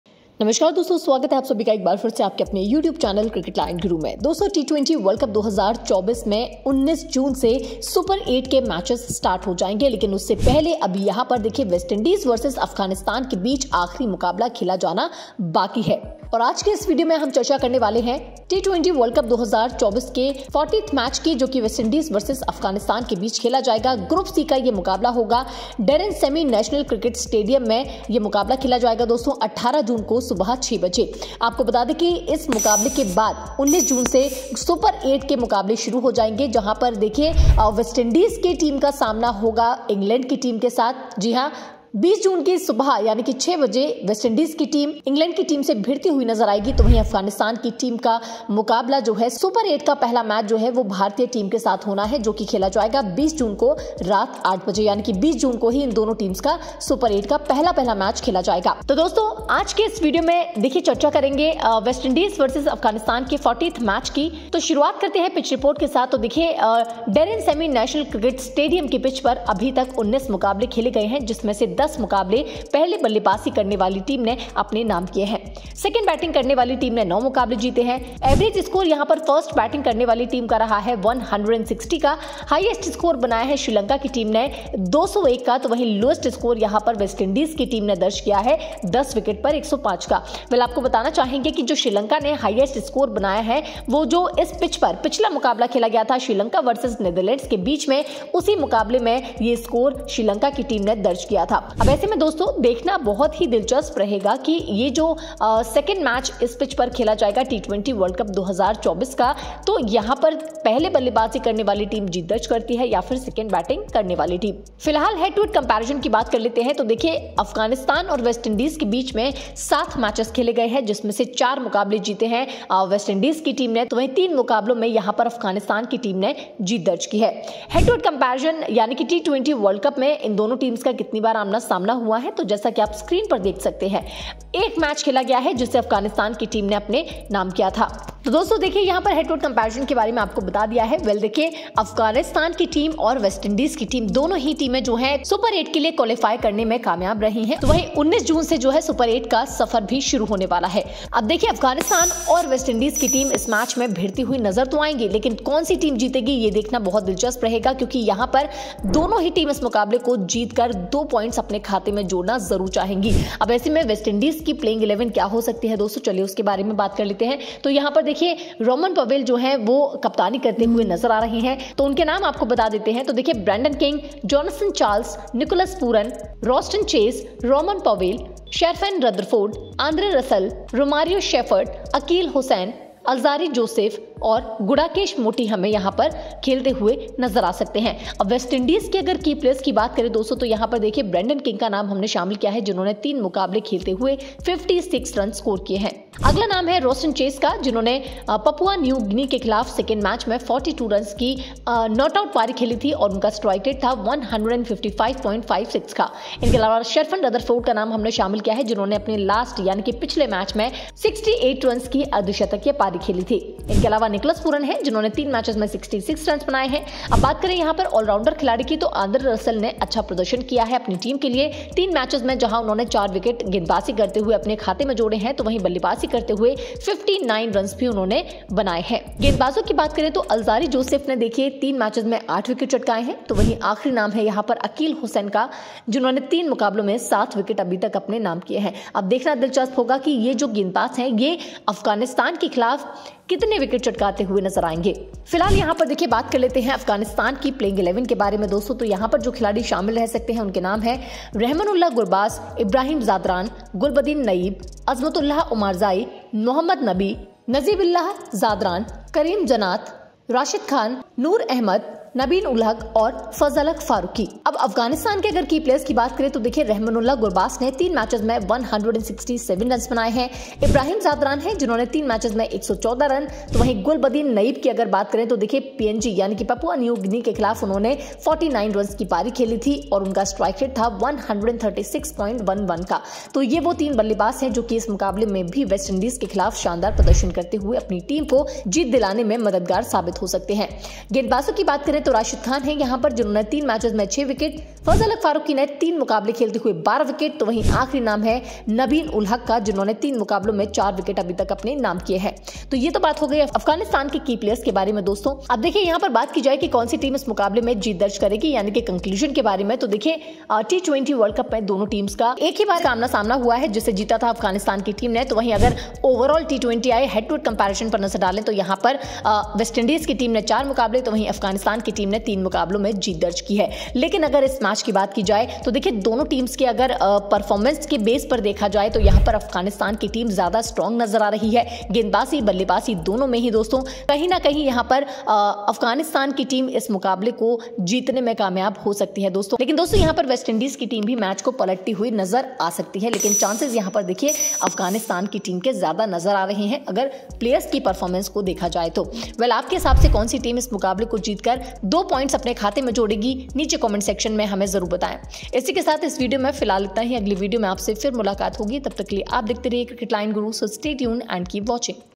West Indies vs Afghanistan 40th Match Prediction, T20 WC 2024 Group C WI vs AFG Dream11 Prediction
All the important information about West Indies vs Afghanistan 40th Match,  like Playing 11, Pitch Report, Key Players, and Dream11 team, you will get in this video so that you can make better predictions with a better analysis.